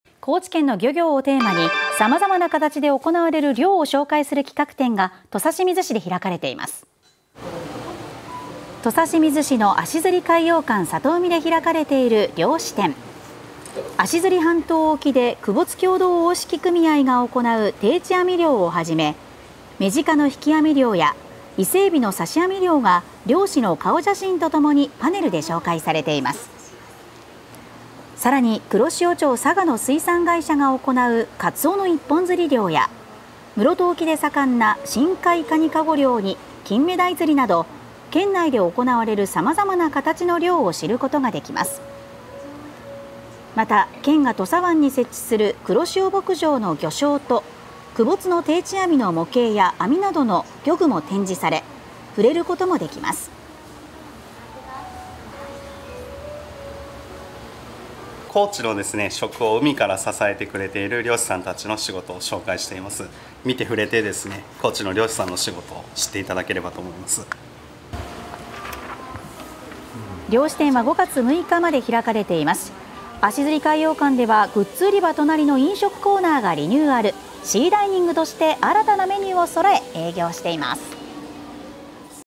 「見て、触れて高知の漁師の仕事を知って」足摺海洋館SATOUMIで“漁師展”開催
“高知県の漁業”をテーマに、様々な形で行われる漁を紹介する企画展が、高知県土佐清水市で開かれています。

土佐清水市の足摺海洋館SATOUMIで開かれている「漁師展」。足摺半島沖で窪津共同大敷組合が行う『定置網漁』をはじめ、メジカの『引き網漁』やイセエビの『刺し網漁』が漁師の顔写真と共にパネルで紹介されています。さらに黒潮町佐賀の水産会社が行う『カツオの一本釣り漁』や、室戸沖で盛んな『深海カニ籠漁』に『キンメダイ釣り』など県内で行われる様々な形の漁を知ることができます。

また、県が土佐湾に設置する“黒潮牧場”の漁礁と窪津の定置網の模型や網などの漁具も展示され、触れることもできます。

（足摺海洋館SATOUMI　新谷諭崇さん）
「高知の食を海から支えてくれている漁師の仕事を紹介しています。見て、触れて高知の漁師の仕事を知っていただければと思います」

「漁師展」は5月6日まで開かれています。足摺海洋館ではグッズ売り場隣の飲食コーナーが3月15日にリニューアル。「Sea Dining」として新たなメニューを揃え営業しています。